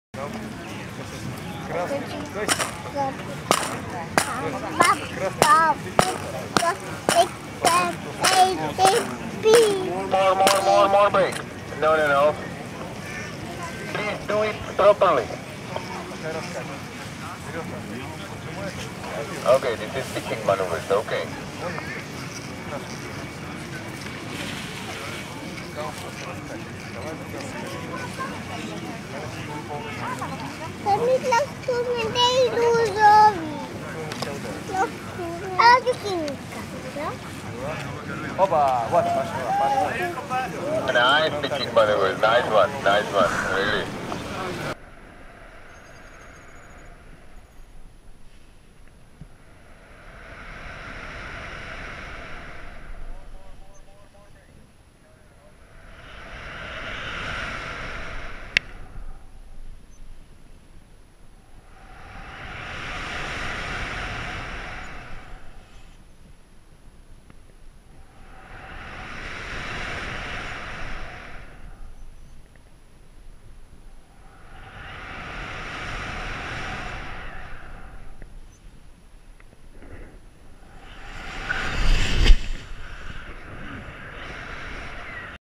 SIV - Pitching Control
التدريب ع بعض الحركات من دورت الـSIV للطيران الشراعي مع المدرب توماس من التشيك في اولدنيز تركيا اكتوبر 2014